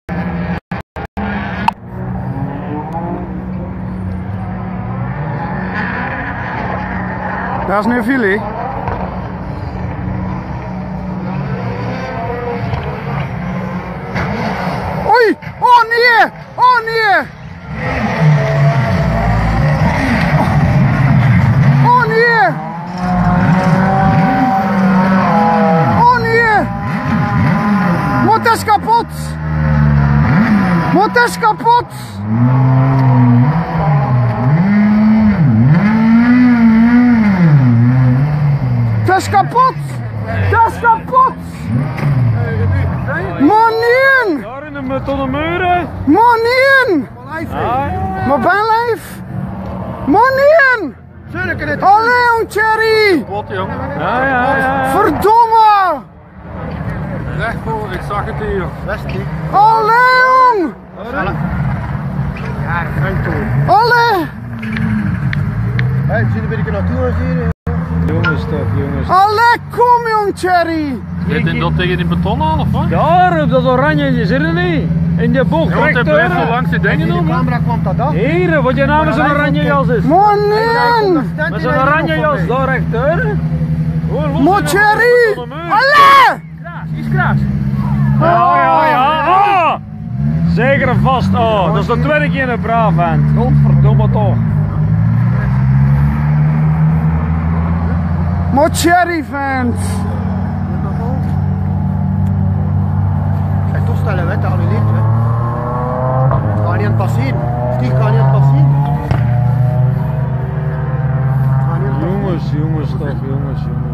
WRC Rally Sweden 2017 - Thierry Neuville CRASH
Video credit: Vincent Verstraete